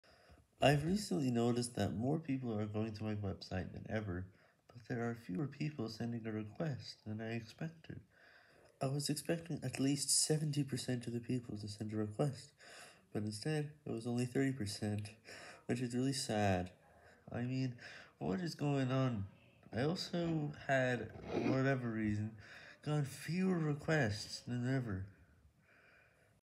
More people are visiting my website. But…
There were not as many requests even though I was getting more visits.